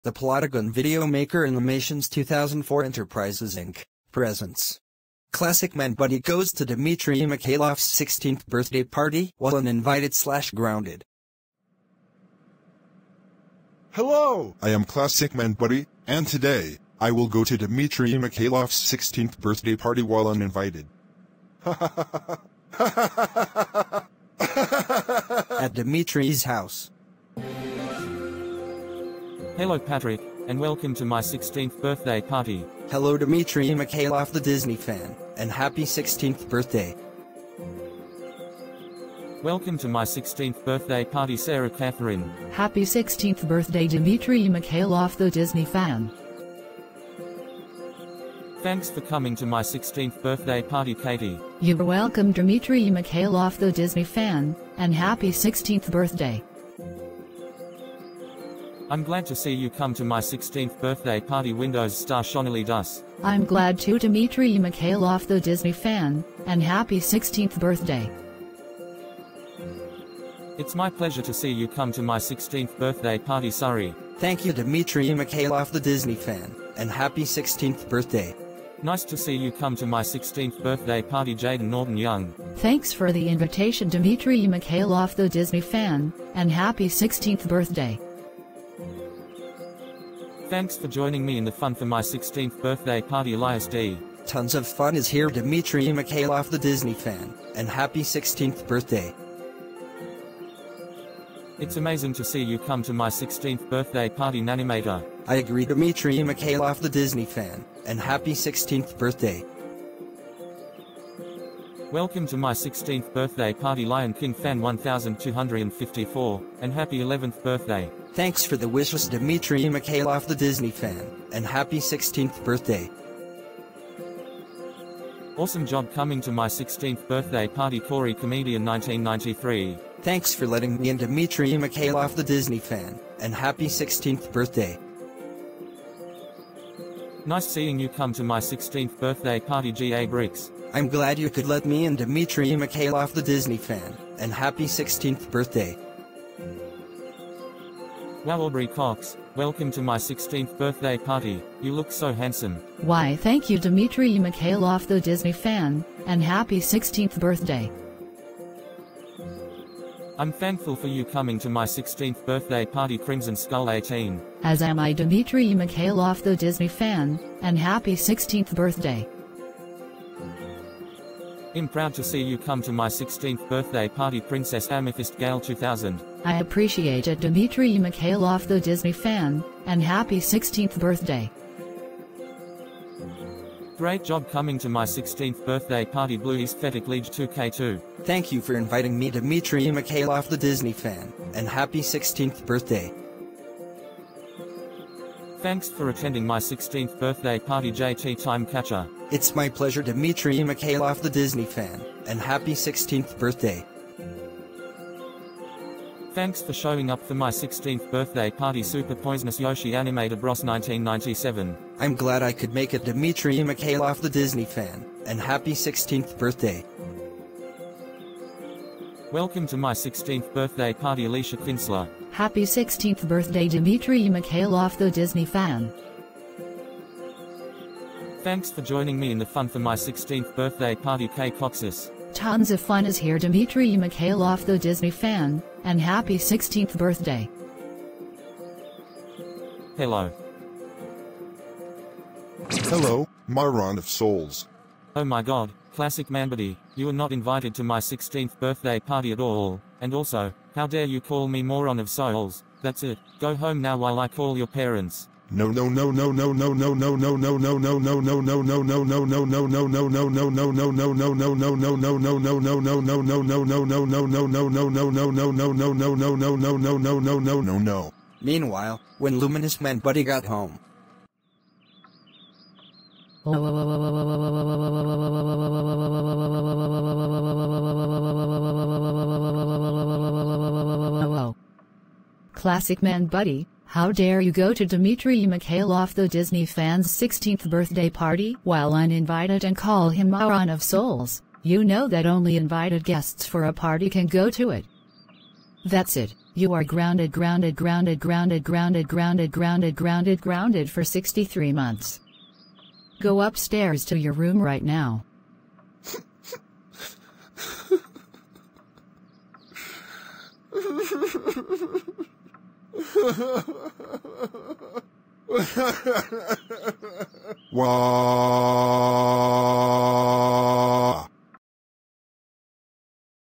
Classic Manbuddy Goes To Dimitriy Mykhaylov’s 16th Birthday Party While Uninvited/Grounded
This video is a video special for @DimitriyMykhaylovDisneyFan2006’s 16th Birthday

Anyways

Me

Sara Catherine

Katie

SurreyTheGoAnimatorFan2K4